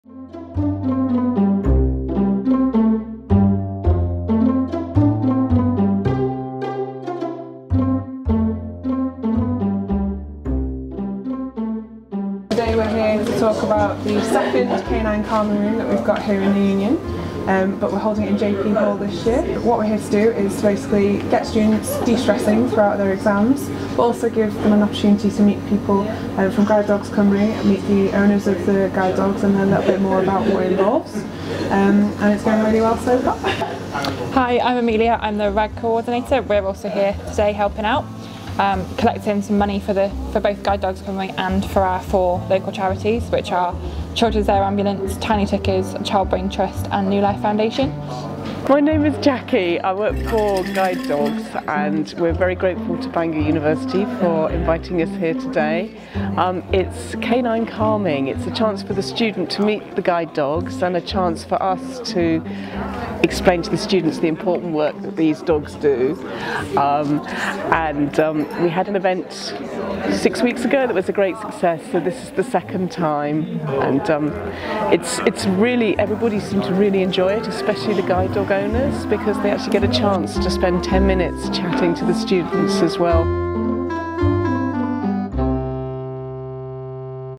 Canine Calming Room 2 - Bangor University
Canine Calming Room 2 in John Phillips Hall – an event organised by Bangor Students' Union, Bangor University RAG and Guide Dogs Cymru.

Bangor University students get a chance to meet local guide dogs and their owners in the calming canine room. It's a chance for the students to de-stress during the busy exam period and a chance for the guide dog owners to chat to the students about the amazing work that the guide dogs do for them.